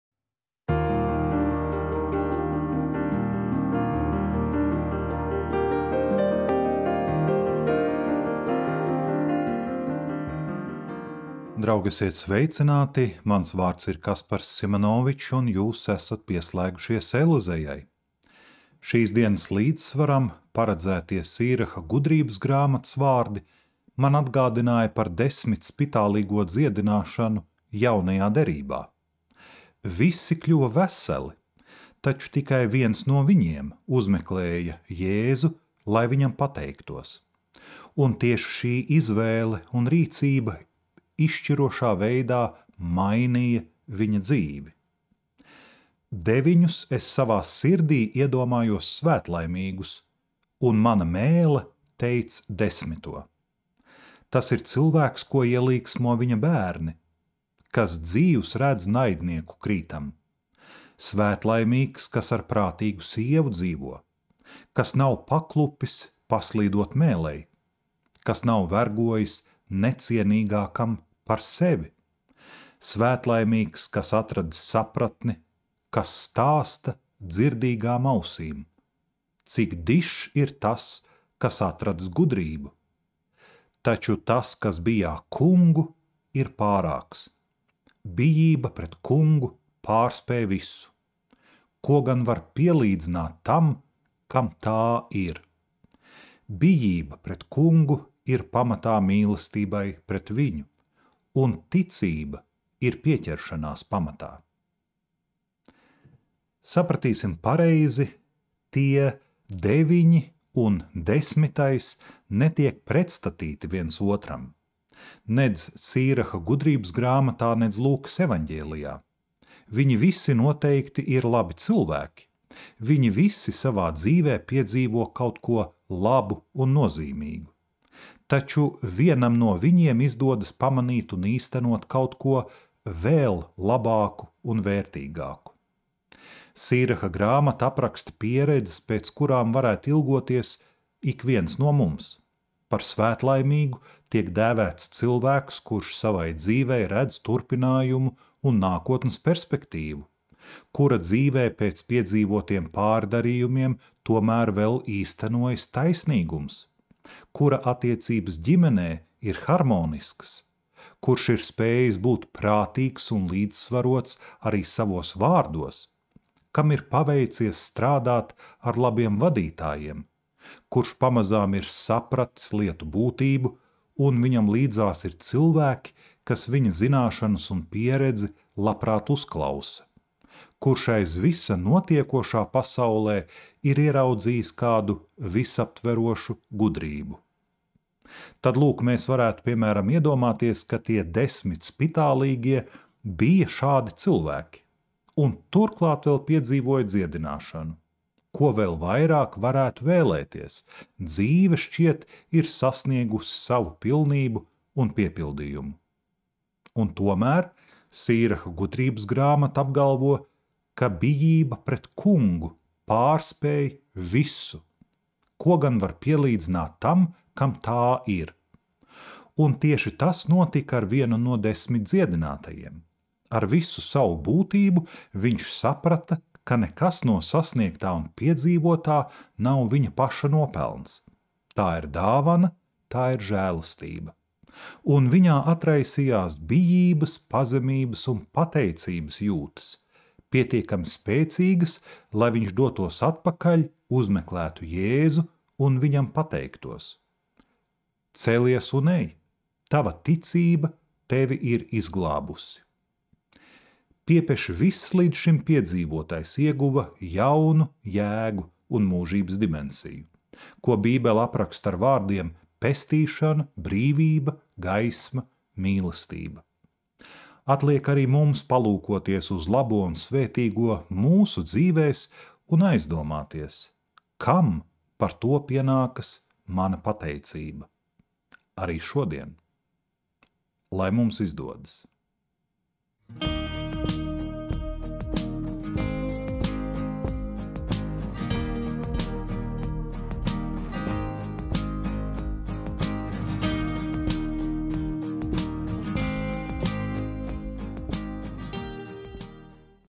Dienas līdzsvars - Sīraha 25,7-12
Šīs dienas līdzsvaram paredzētie Sīraha gudrības grāmatas vārdi man atgādināja par desmit spitālīgo dziedināšanu Jaunajā Derībā. Visi kļuva veseli, taču tikai viens no viņiem uzmeklēja Jēzu, lai viņam pateiktos. Un tieši šī izvēle un rīcība izšķirošā veidā mainīja viņa dzīvi.

Pateicamies par atbalstu Noētiskās veselības biedrībai ELIZEJA!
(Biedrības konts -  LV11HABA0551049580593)